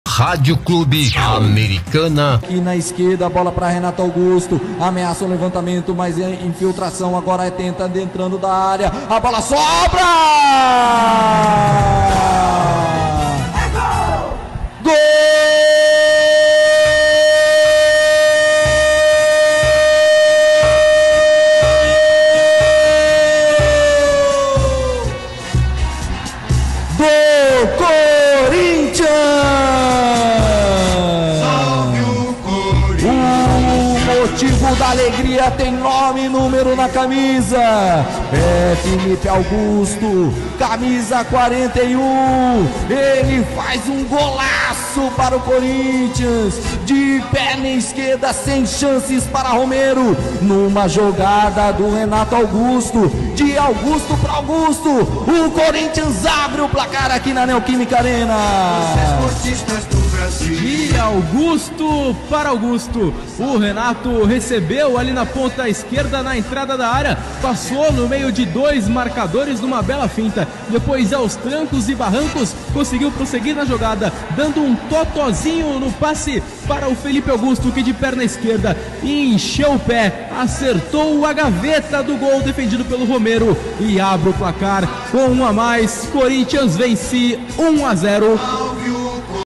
SC Corinthians 1 x 0 Universitario - 1ª partida da fase Playoffs da Copa Sulamericana (12-07-2023)
Confira a narração do gol da vitória do Sport Club Corinthians Paulista sobre o Club Universitario de Deportes pela 1ª partida da fase Playoffs da Copa Sulamericana

📻: Rádio Clube Americana 580 AM / O Liberal TV

🎤: Bruno Martins, Lucas Ardito, Gustavo Tomazeli , Rebecca Fortunato e Gustavo Belofardi

📷: SC Corinthians Paulista